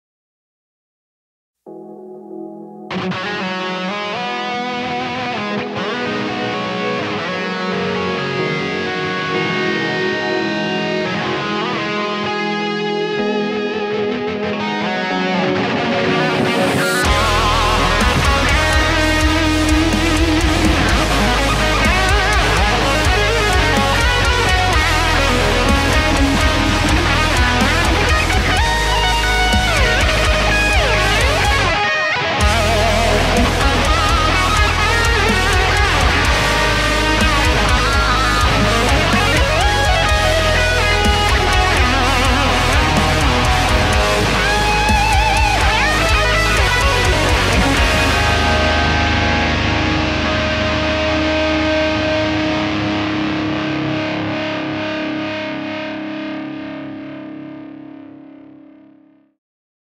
Jeremy Pringsheim KieselSoloContest Entry
This is my entry for the Kiesel Solo Contest //

I'm not a shredder so I tried to make it a bit more song-like and less straight up madness because I can't play like some of you crazy people out there haha..!! I hope you like my approach!